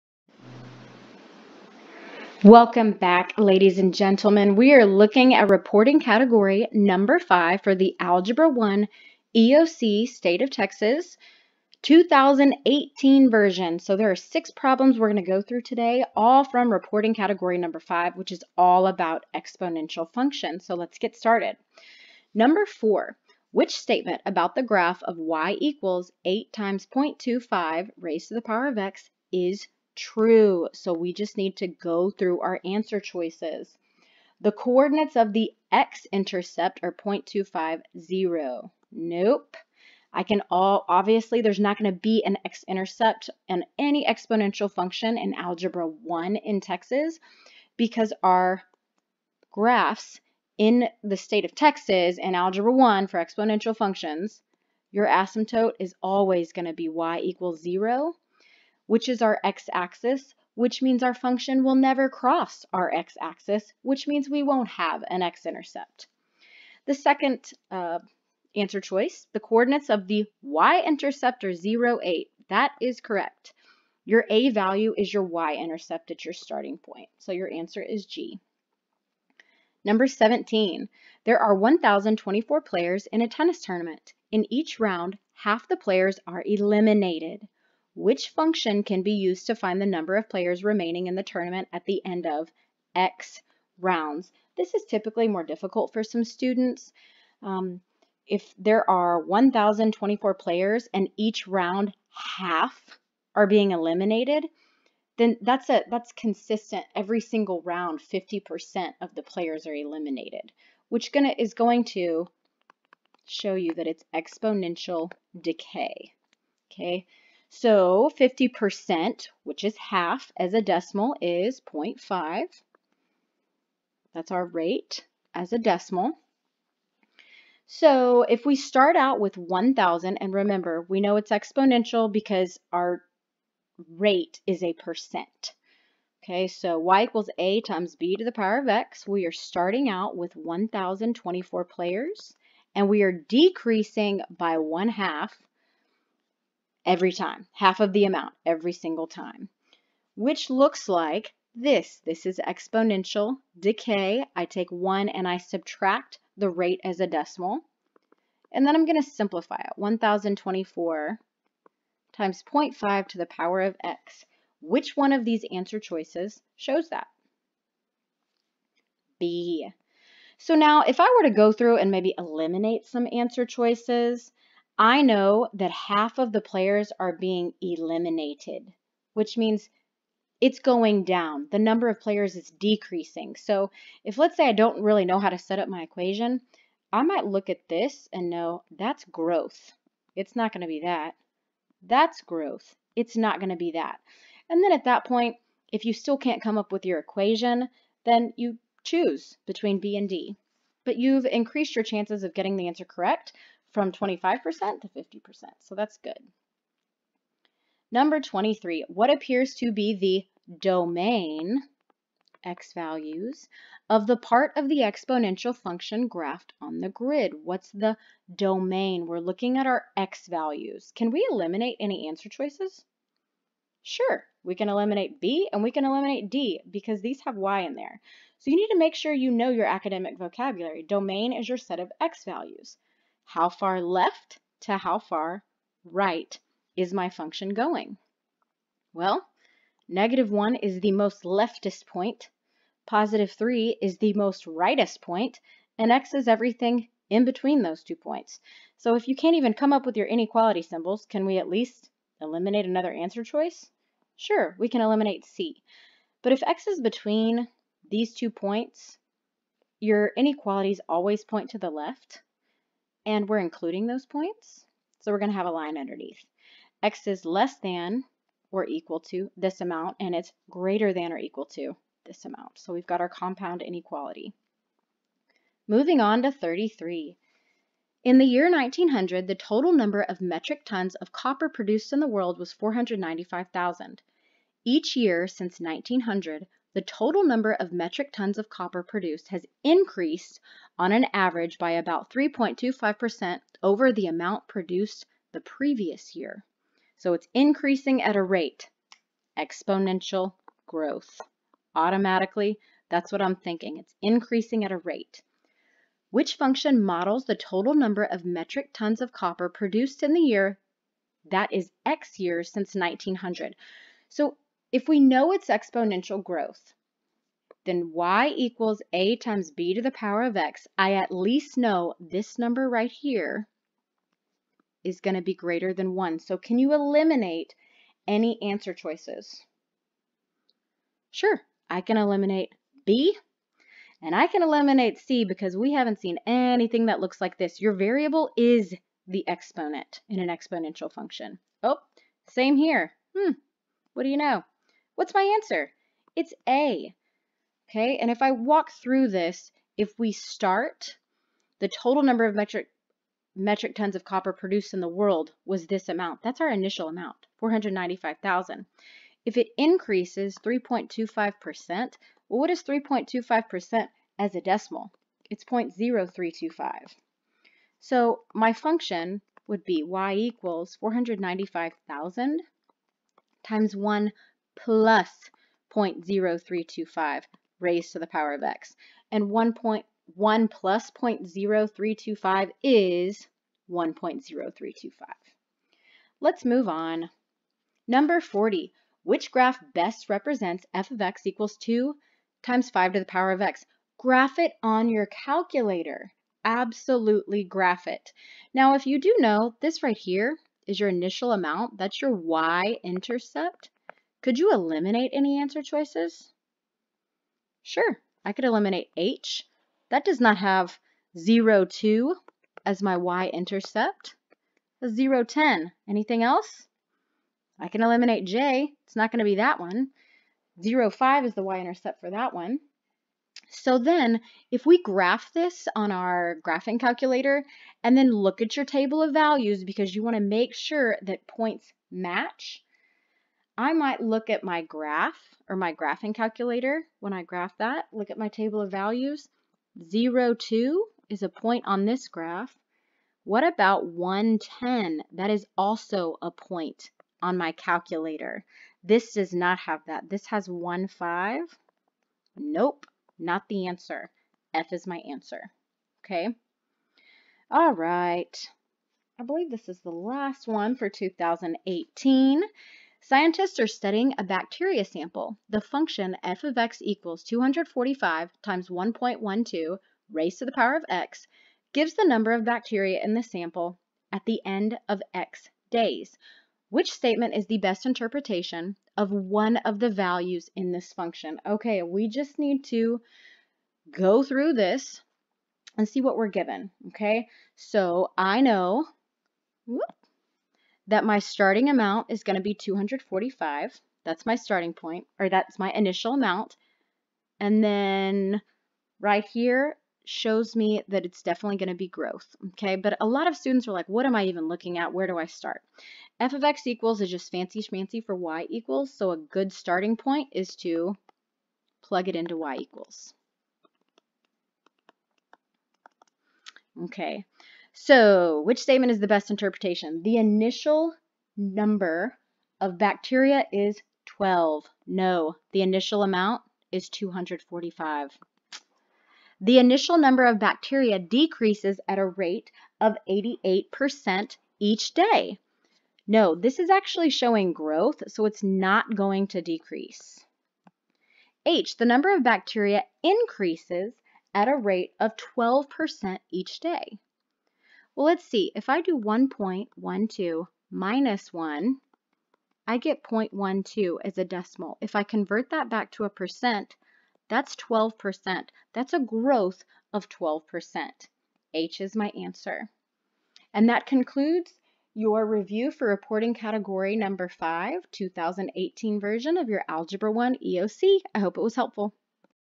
Algebra 1 Reporting Category #5 Review 2018 Version
Work through all of the problems from the 2018 version of the Algebra 1 End of Course Exam that deal with concepts in Reporting Category #5.  Learn helpful tips and calculator strategies to pass the EOC.

All previous EOC tests are on the TEA website for public access.

Download the notes to go along with the video on my teachers pay teachers